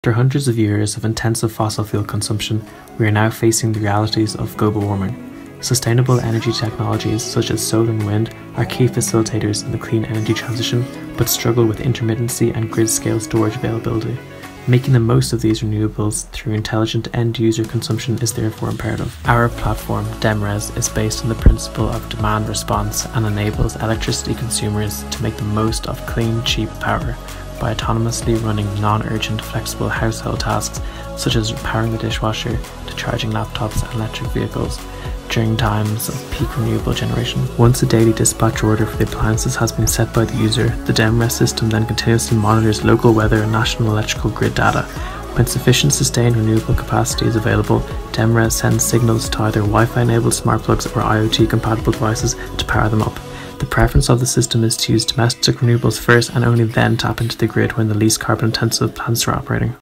Welcome to DemRes
DemRes - Application video for the RedBull Basement University competition 2020.

Sustainable energy technologies such as wind and solar are the key facilitators of the clean energy transition, but these suffer with intermittency and a lack of grid scale storage. Making the most of these renewables while they are available is therefore crucial. DemRes enables us, as energy consumers, to intelligently schedule our electricity demand over the course of a day and match the supply of these renewables, thus reducing our electricity cost and carbon footprint. The user simply lets the DemRes app know what appliances they would like to power up that day, and the system then continuously monitors domestic renewables production, such as rooftop solar PV, and national grid data to autonomously activate them at times of peak renewables generation.

Ben Ralph and Tadhg Crowley
Ireland

(Side note: All stock video and music included in this production were sourced from Pexels and Epidemic Sounds respectively. These are free to use and have been adequately licensed as such.)